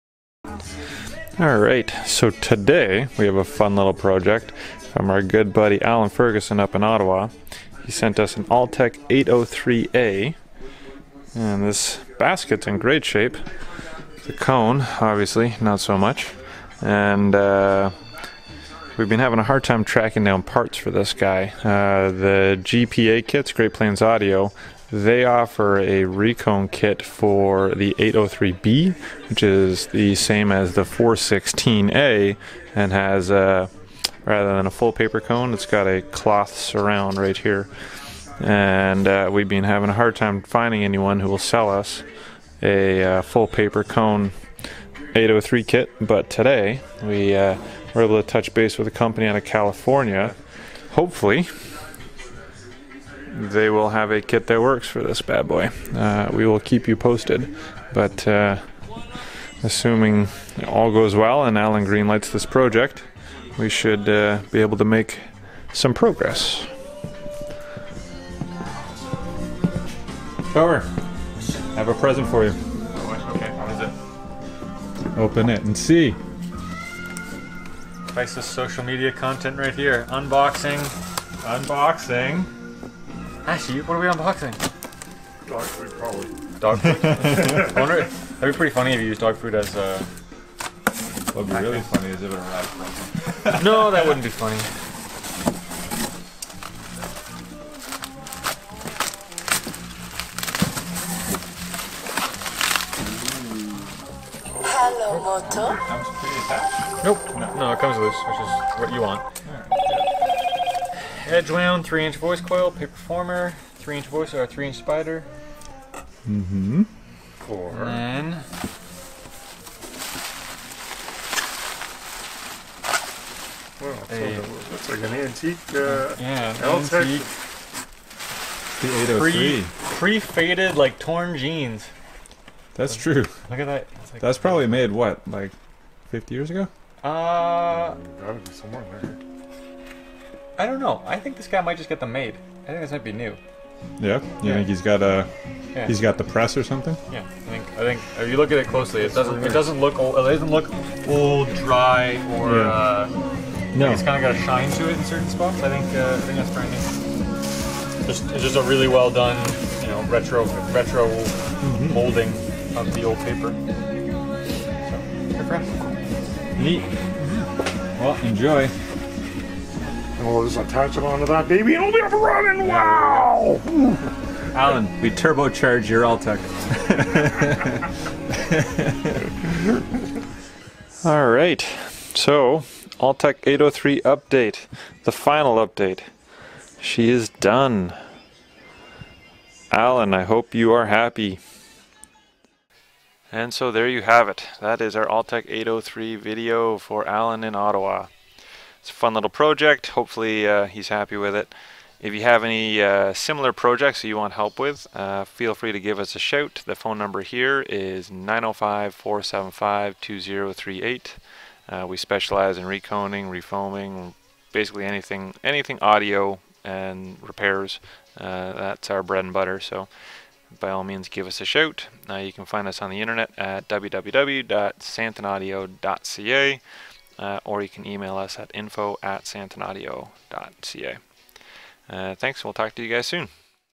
Altec 803A Recone Project - Full Paper Cone
We received a beautiful Altec 803A basket from a client up in Ottawa, Ontario (Canada) that was in desperate need of some love - After a bit hunting online, we were able to track down a kit and get this driver fully restored to original spec and sounding great.

If you are looking for help restoring your vintage speakers definitely give us a shout - We operate out of Toronto and would love to help you bring your speakers back to life - /

Update - January 8th 2019 - Note from client:

"Received the shipment yesterday (Sunday) and just wanted to note that you have set a new standard in quality service. The work on the woofer, the packing of this delicate instrument, the speed of delivery can only be described as exceptional. Thank you Mickey and Chris!

I tested it this morning and it works perfectly.  Thanks again for a perfect restoration.

Now to find a mate for this museum quality speaker!

Cheers,

Allan"